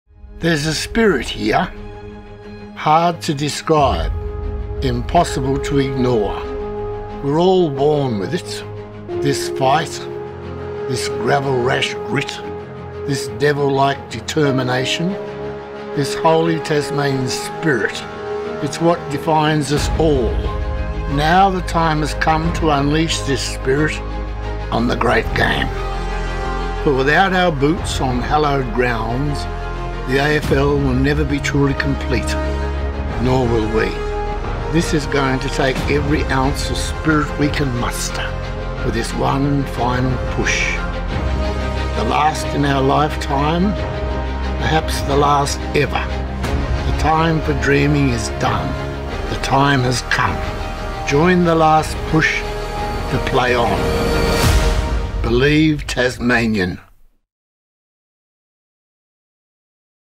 Believe Tasmanian TVC
Now is the time to join the push to play on! Add your name and change the game. Let's get an AFL and AFLW team in Tassie!